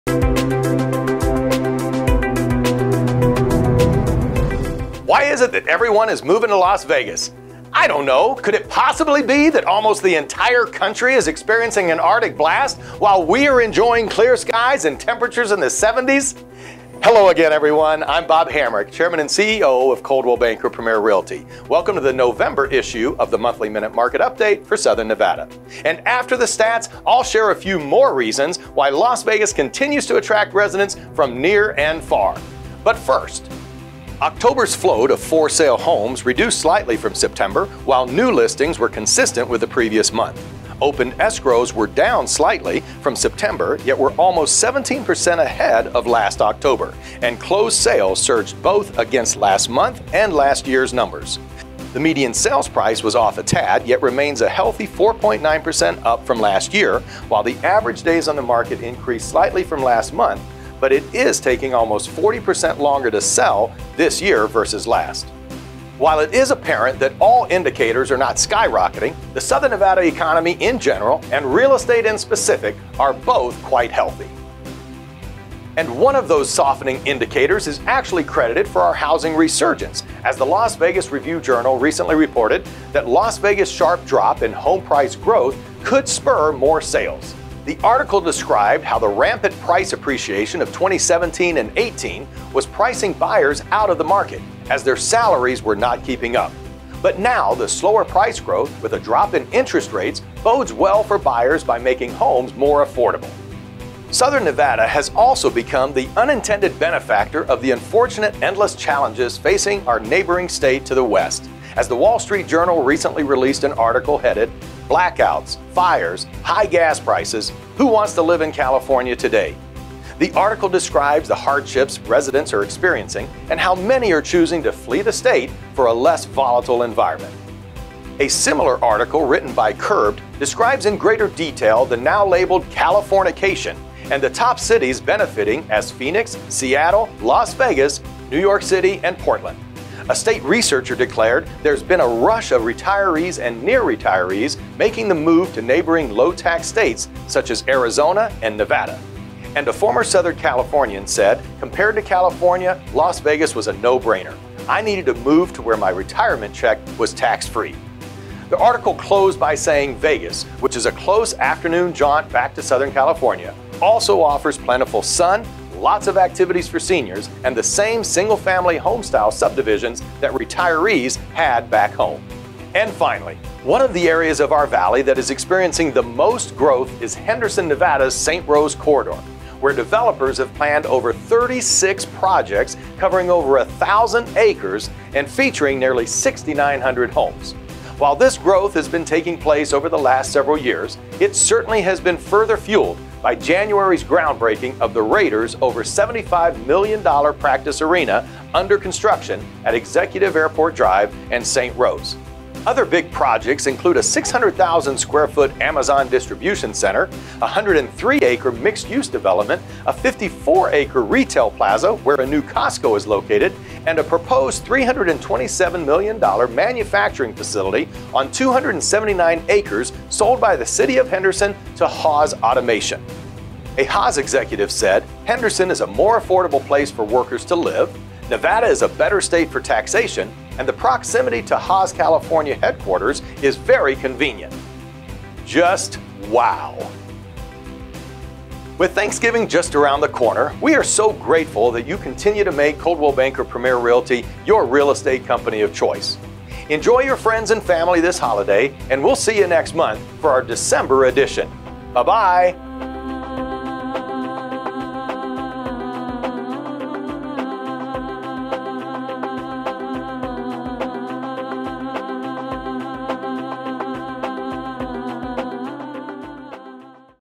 Las Vegas Real Estate Report
Do you have out-of-state relatives that you wished lived here in Las Vegas? If so, watch our monthly real estate report to help you start a conversation about why Las Vegas is the best place to live! In this video, we discuss the factors that continue to grow Southern Nevada's strong economy, including Henderson's St. Rose Corridor where developers have over 36 projects planned.